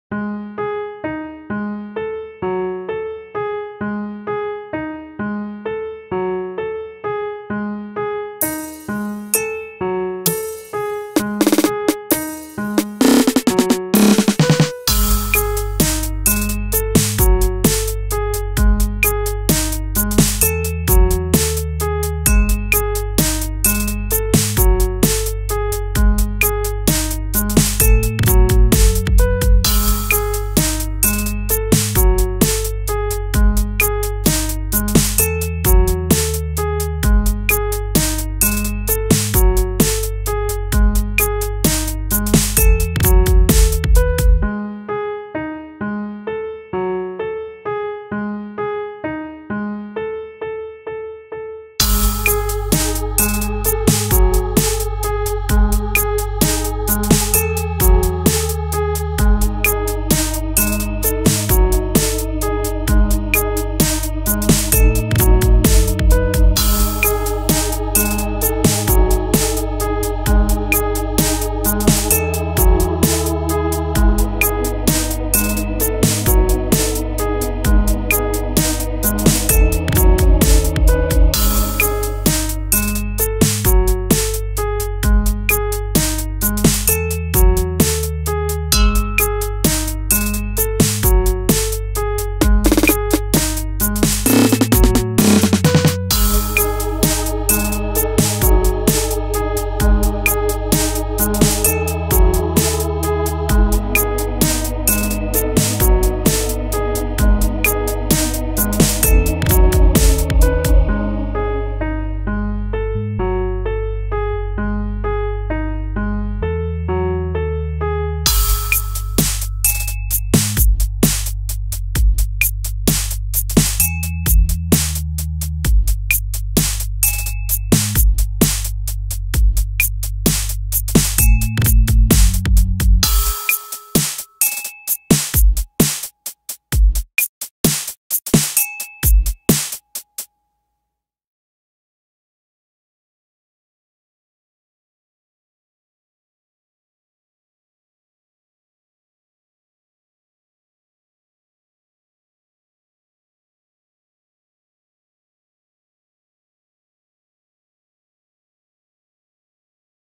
Gangsta Rap Piano Beat FL Studio
My first hip hop style based song. Made in Fl Studio
NOTE: The bass got fucked up as I only listened on my big ass subwoofer with too much bass on. Listening with headphones on I can hear how much I messed up the bass on various parts off the song lol